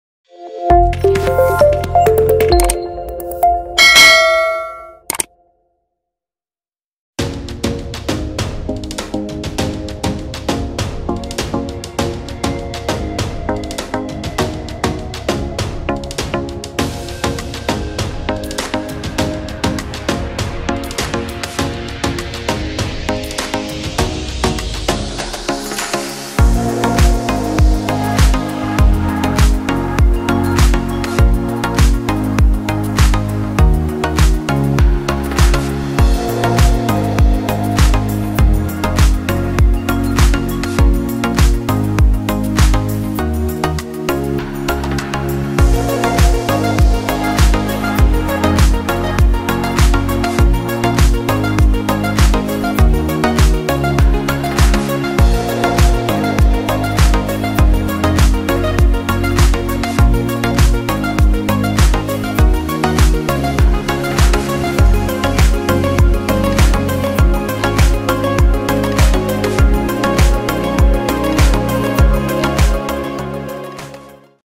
KOIN LUNA ANJLOK DARI HARGA $119-$0.001 DALAM BEBERAPA HARI INI
Channel Berita Youtube Populer adalah Kumpulan video informasi populer  yang disajikan bentuk video